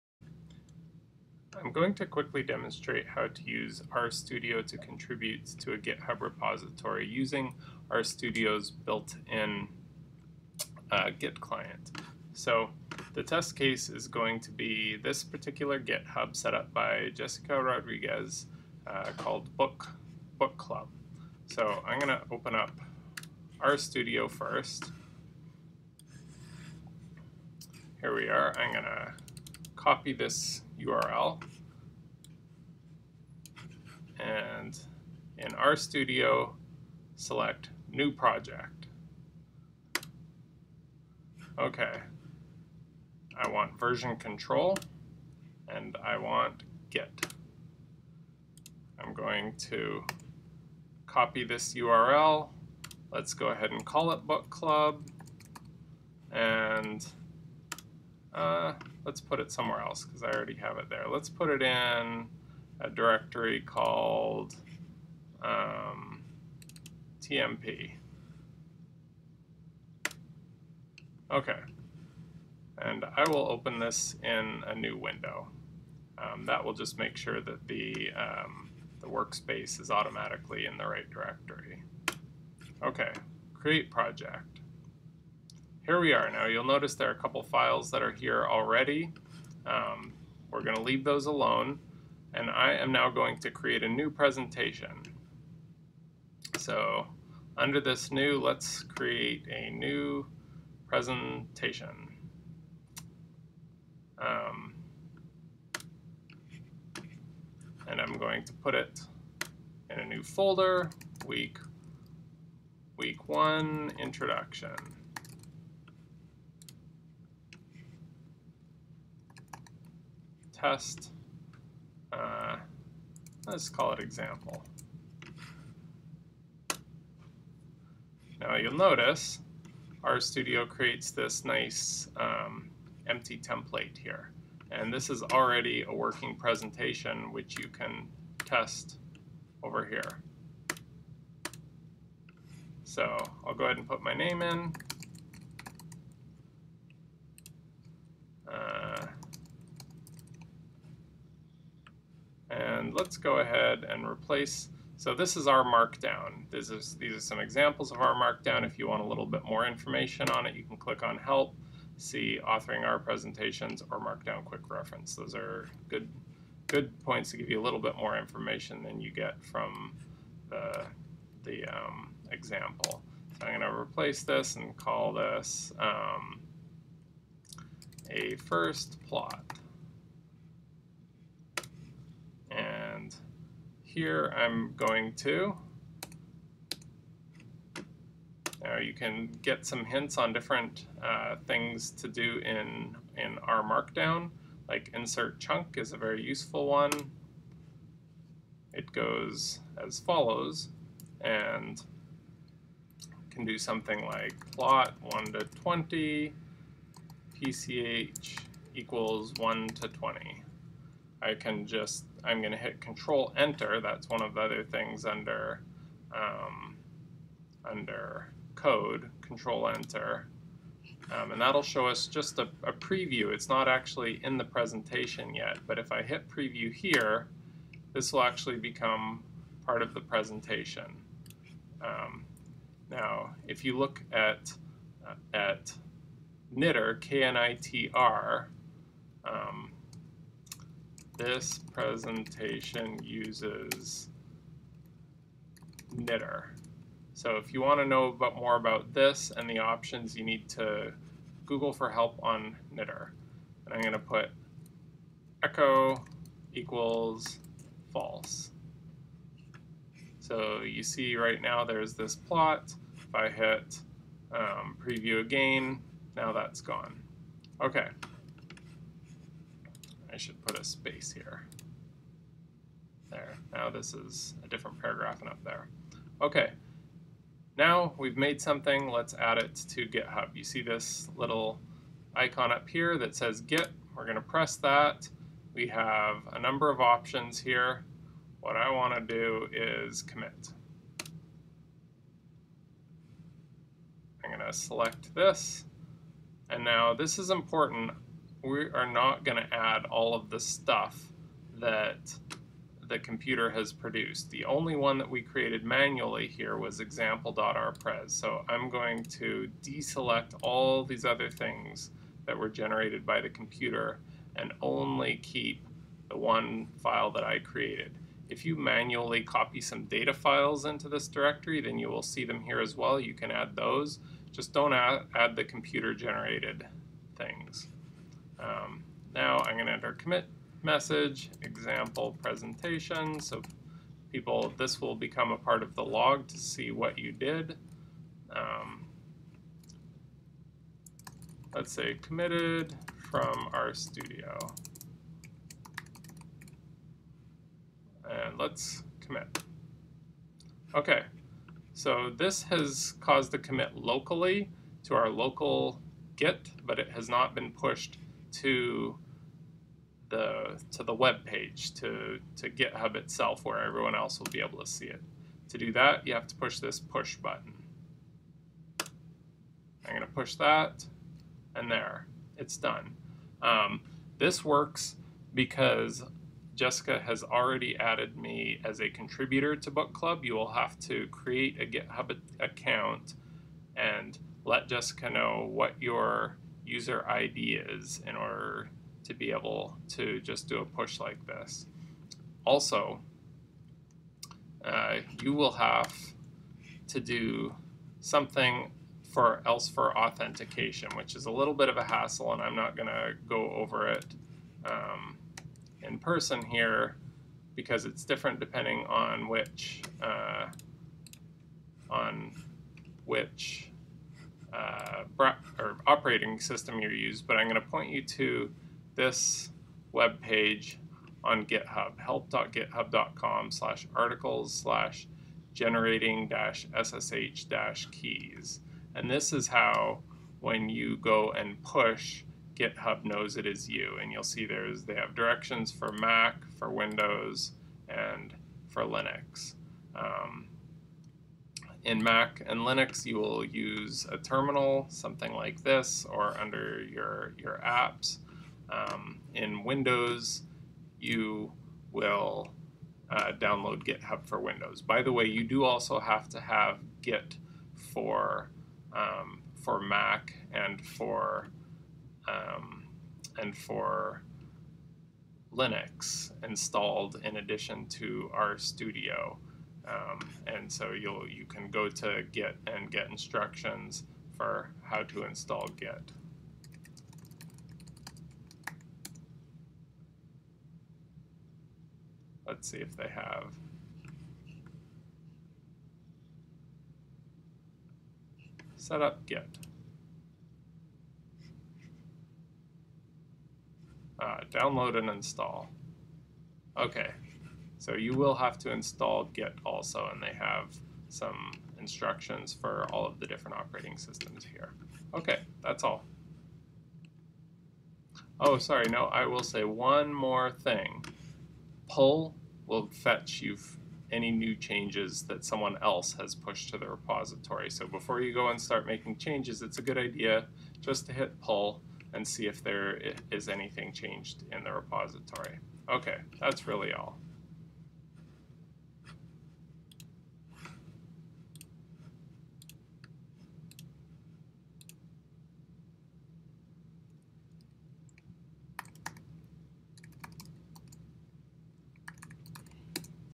Github Rstudio
How to contribute to a Github repository using RStudio